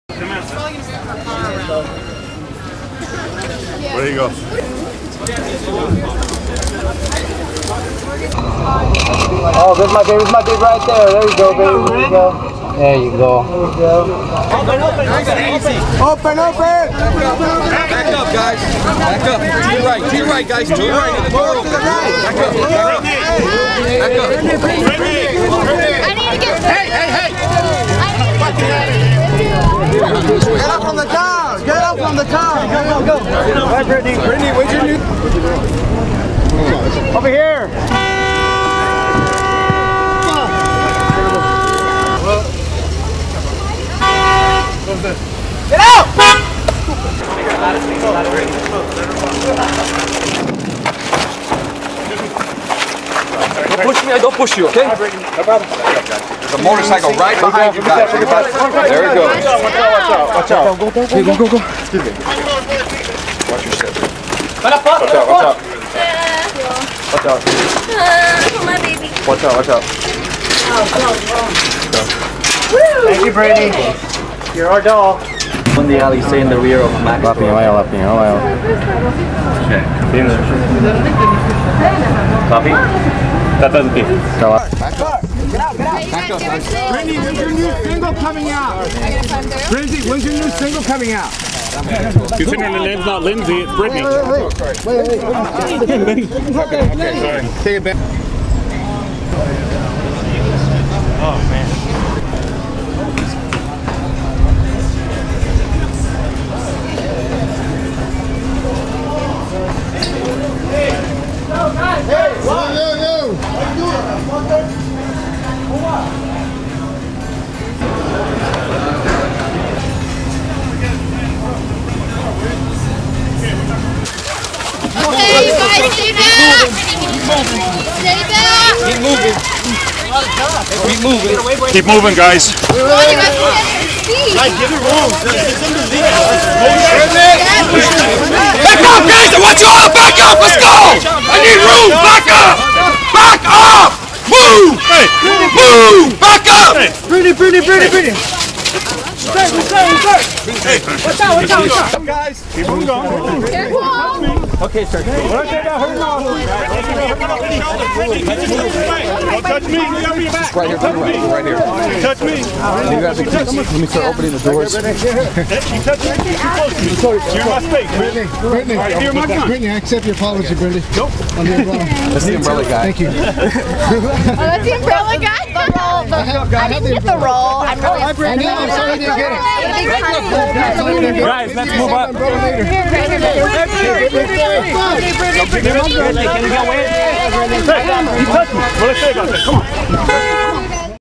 Britney Gets MOBBED While Getting Lunch At Popular Gay Bar The Abbey [2007]
July 23, 2007 - Britney Spears was mobbed by photographers and fans Monday afternoon as she shopping on Robertson Avenue in Beverly Hills.  Her bodyguard went off on paps, telling them to get back!  Brit stopped at Lisa Kline Kids and later she was spotted at The Abbey having lunch.